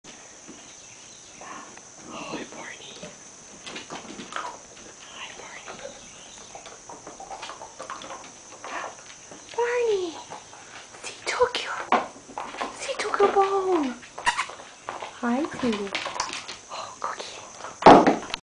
Video of adoptable pet Barney
Barney is an adoptable pet with Delaware Puppy Rescue in Hockessin DE. Please visit their website at